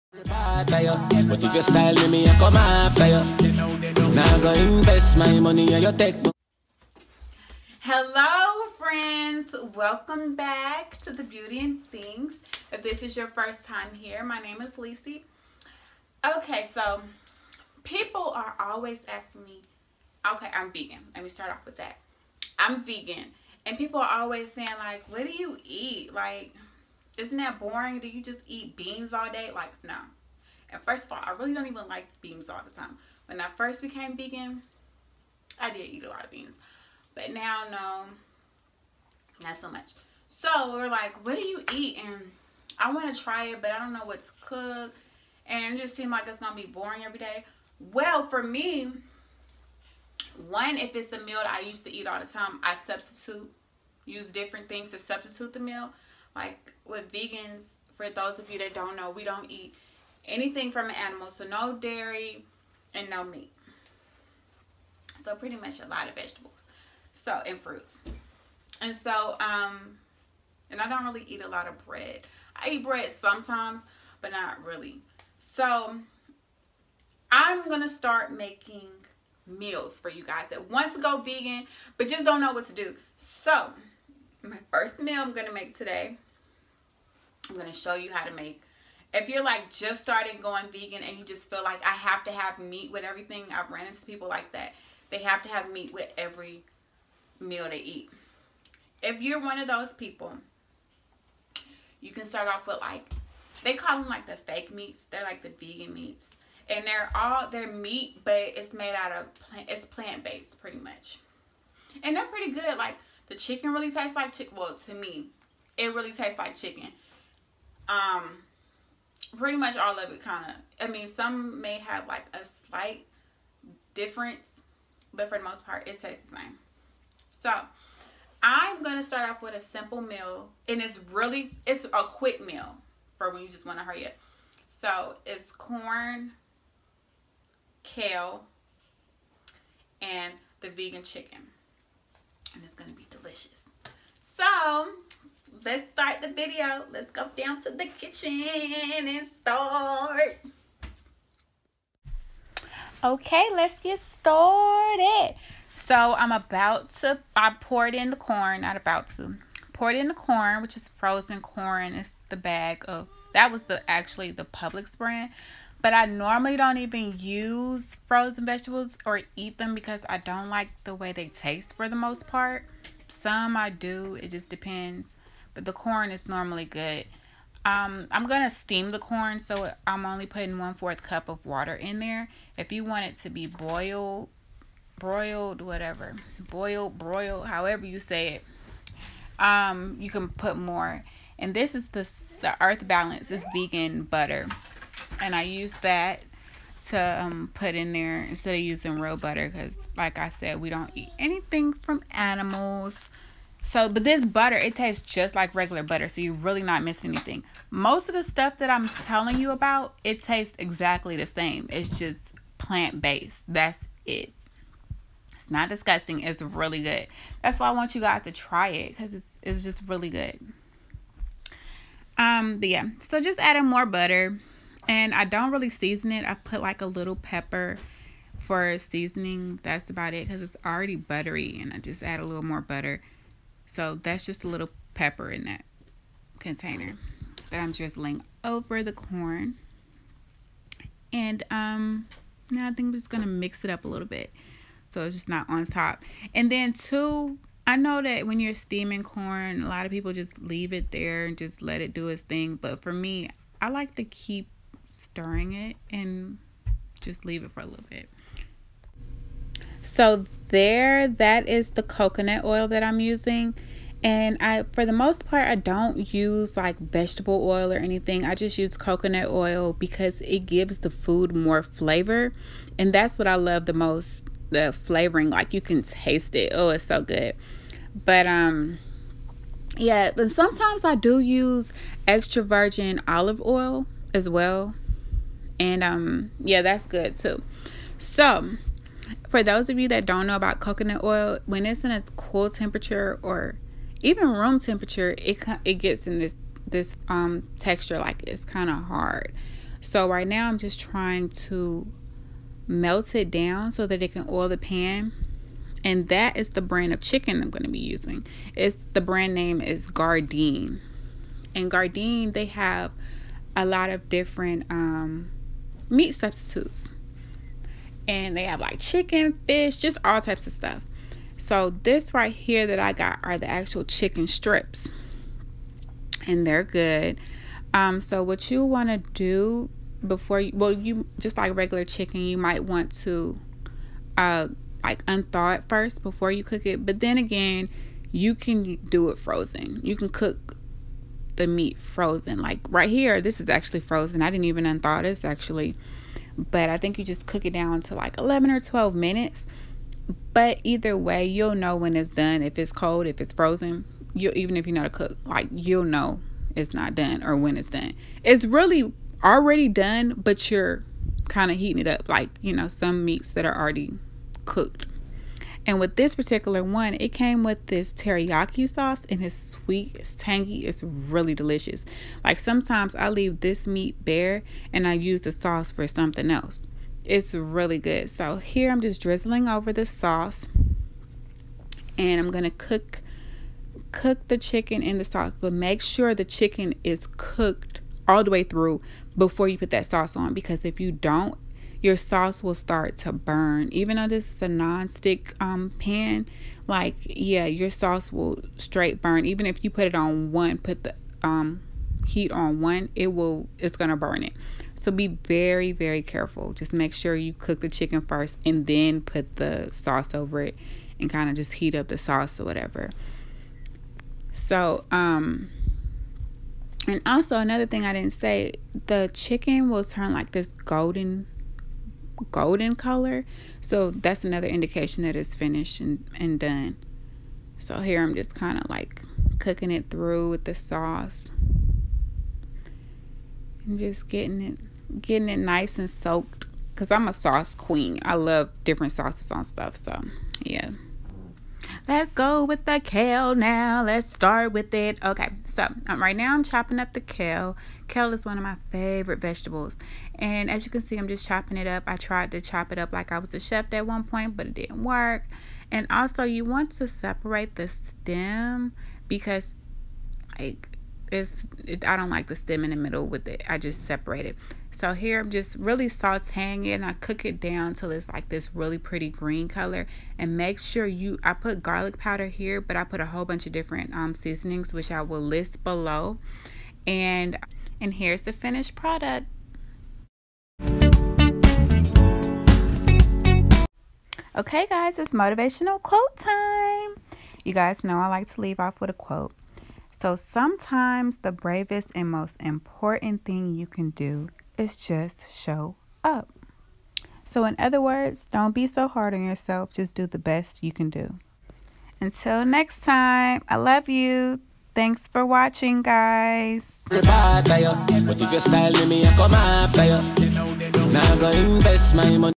Quick/Easy Vegan Dinner
Hi Friends! This is an easy meal for those individuals trying to transition to becoming vegan . Let me know what you think once you try it ! Peace and Love
❤️Lisi

Motivational Quote:
Sometimes the bravest and most important thing you can do is SHOW UP.
~Brene’ Brown